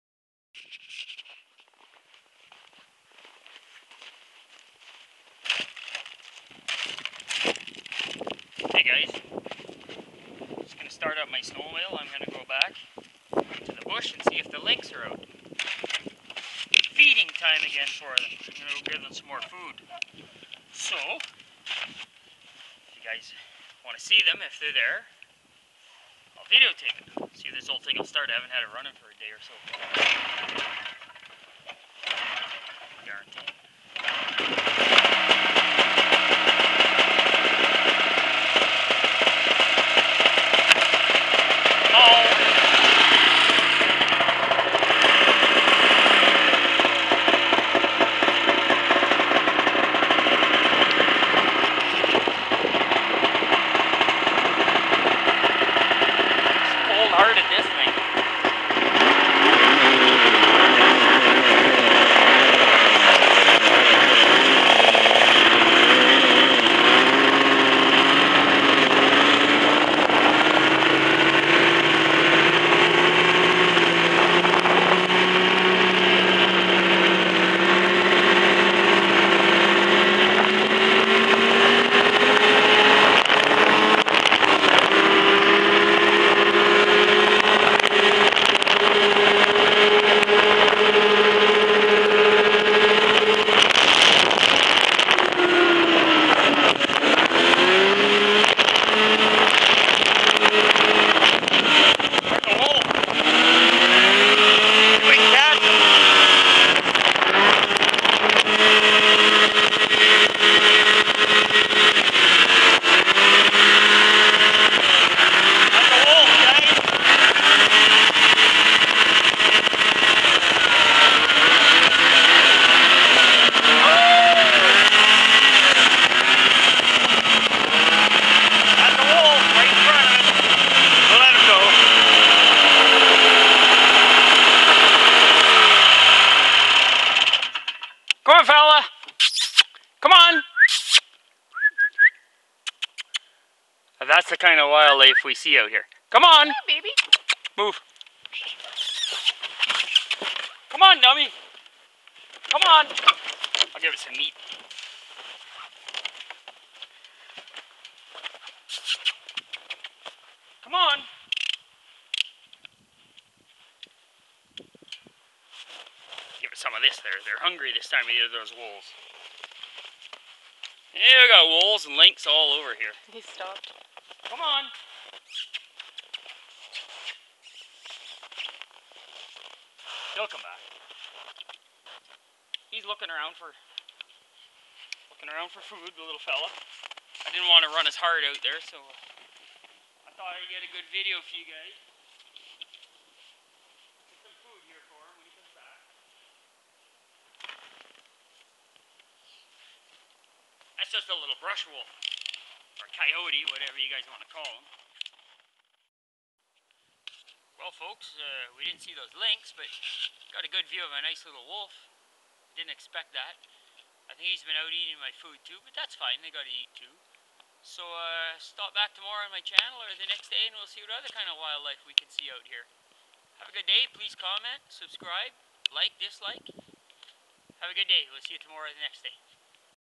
Wolf Encounter avi
I Went Out To Feed The Lynx But Seen A Wolf Instead